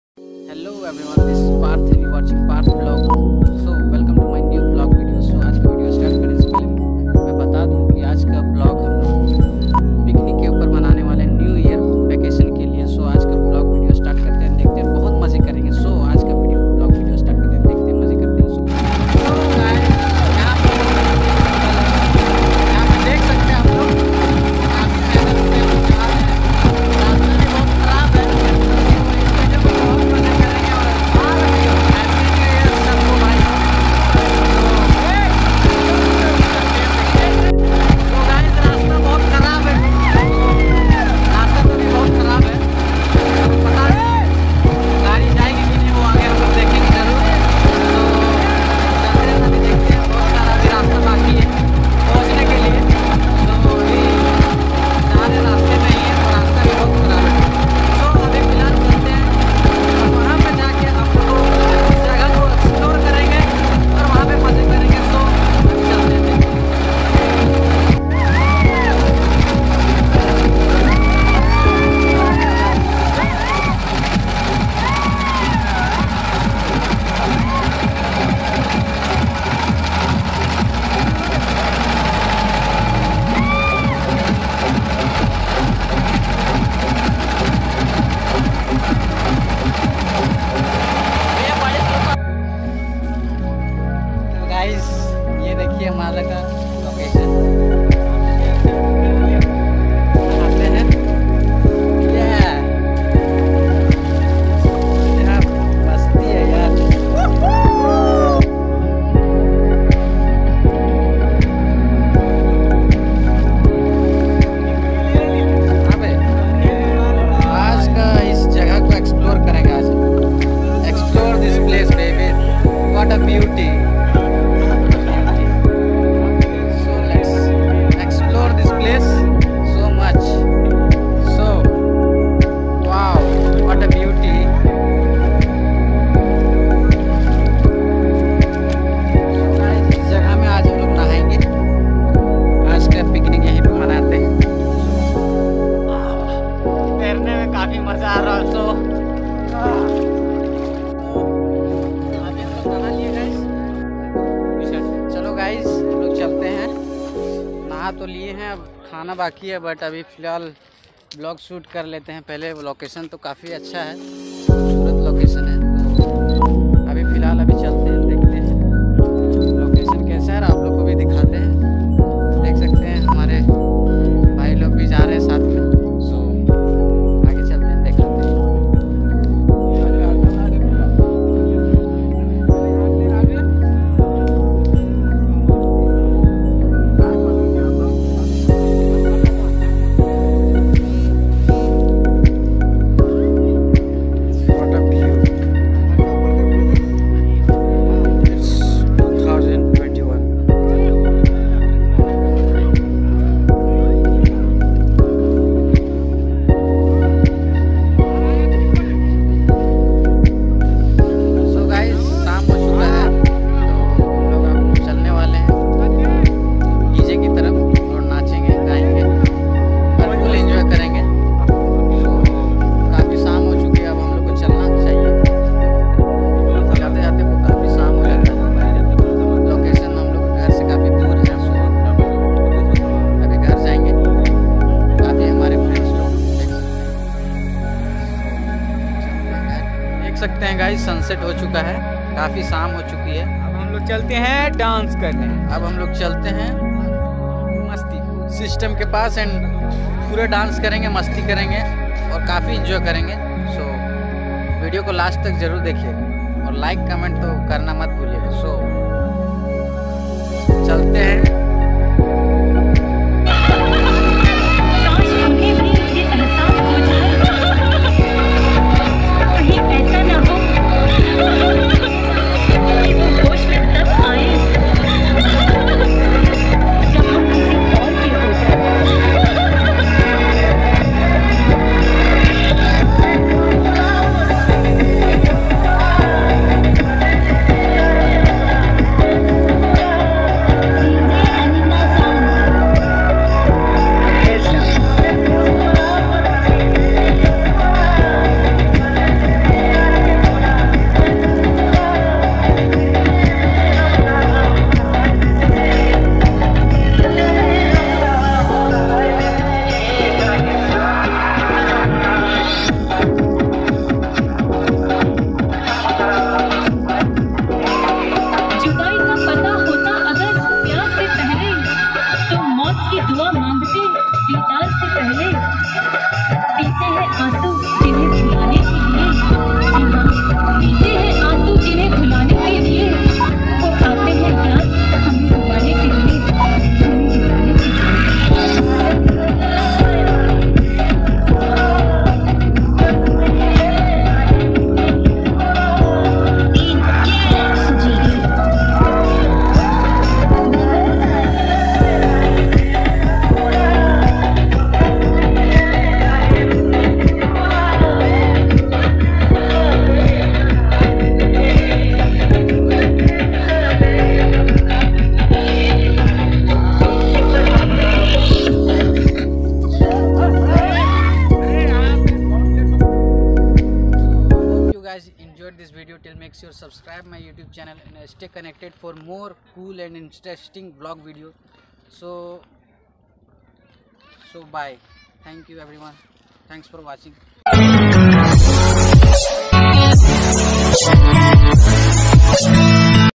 Picnic Vlog With Friends 📸 2021
●This Video is Picnic Vlog with my friends
About Exploring Some New Places In Jharkhand.

So guys enjoy the full video 🤗

Picnic vlogs
Picnic Vlog with my friends
exploring india
exploring jharkhand

This is My First vlog so please support me everyone For Growing My channel❤

Exploring jharkhand

Shot & Edit by - Parth